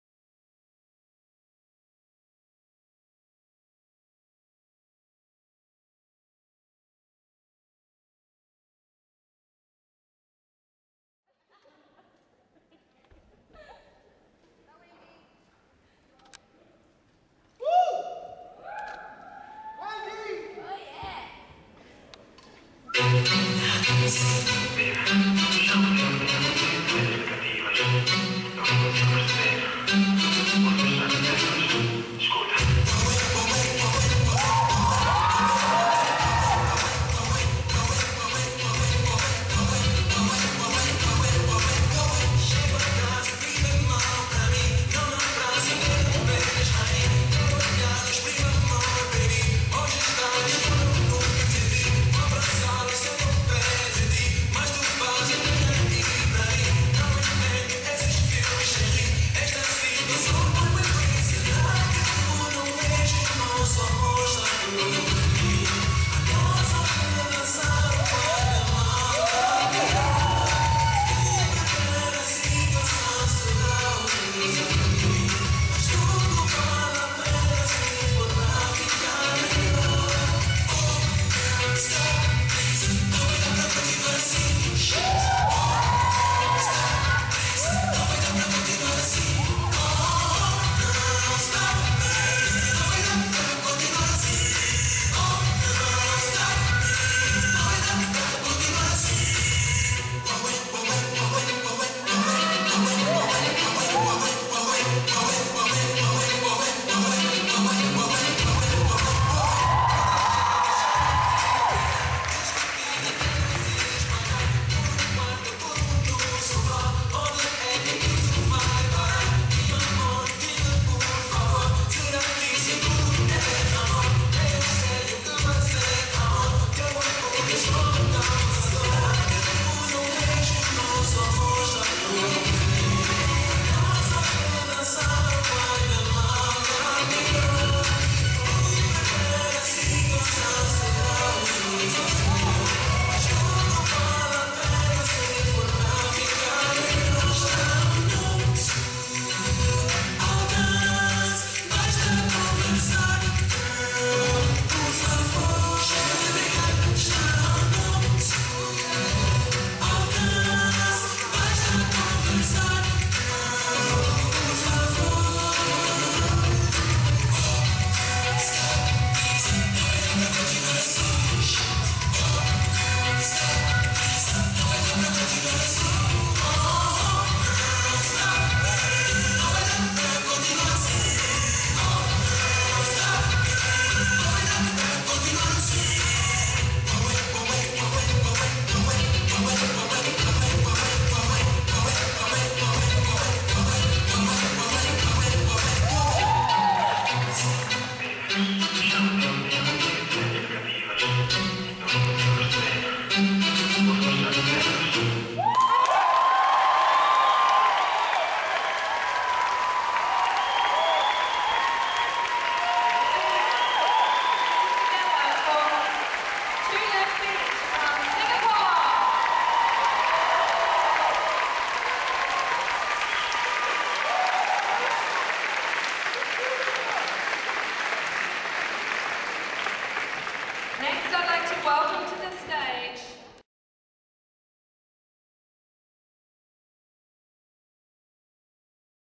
ZOUK - Two Left Feet Dance School - BRAZIL CENTRAL 2010
Brazilian Zouk Lambada

Performed by,
Two Left Feet Dance School from Singapore

at Brazil Central Congress 2010

Alisson Sandi
BRAZUKA DANCE